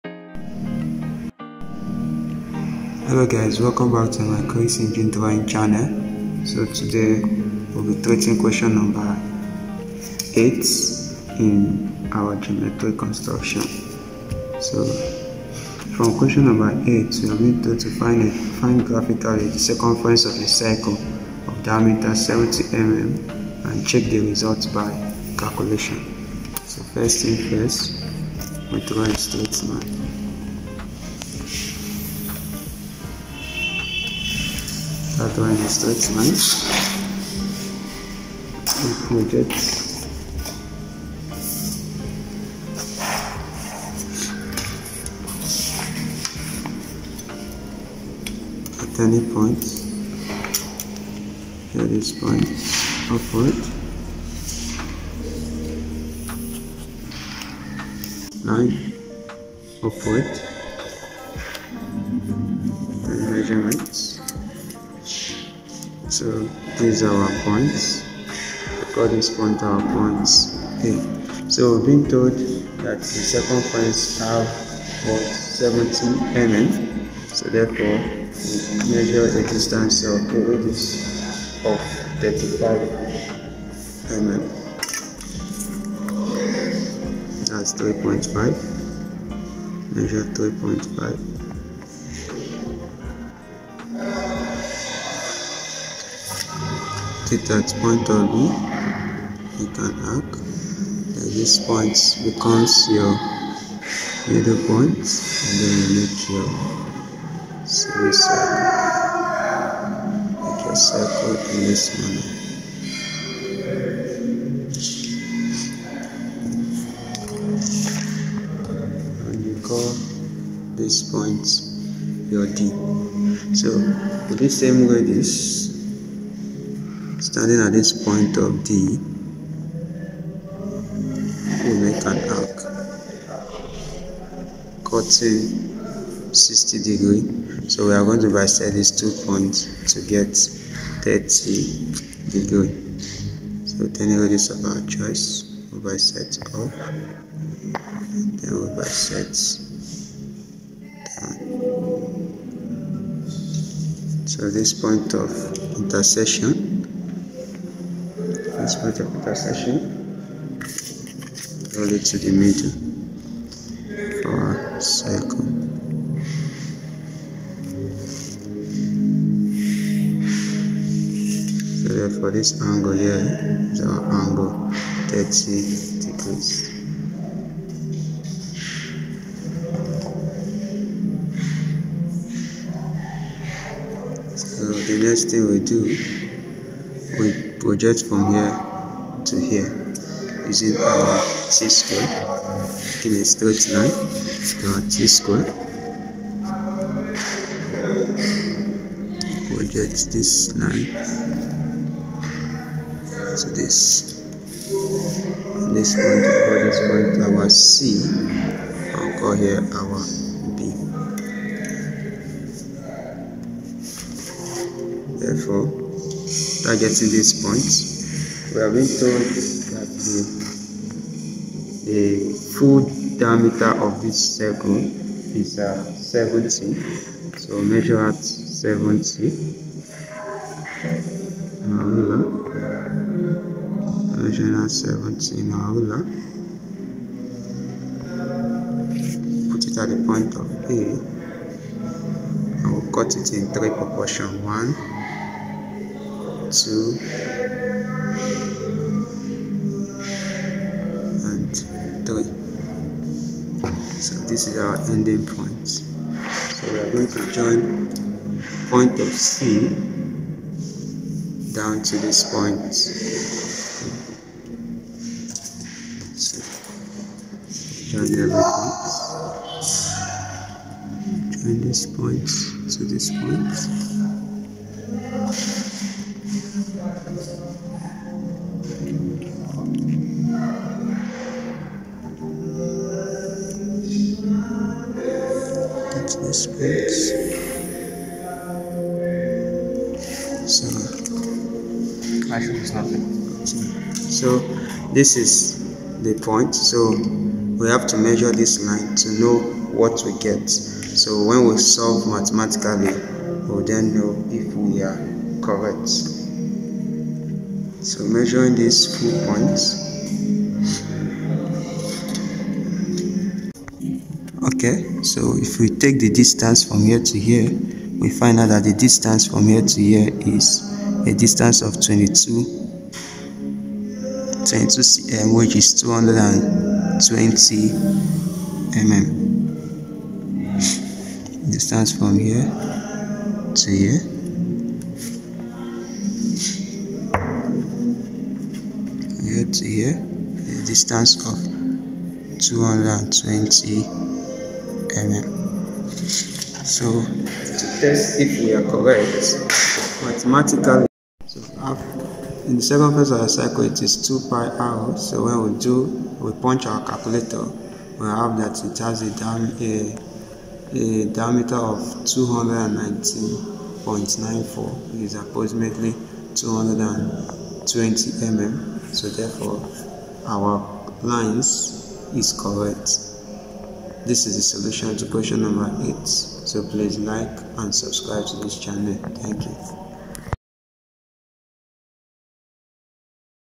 GEOMETRIC CONSTRUCTION. QUESTION NO.8
find graphically the circumference of a circle with diameter 70mm and check the result by calculation.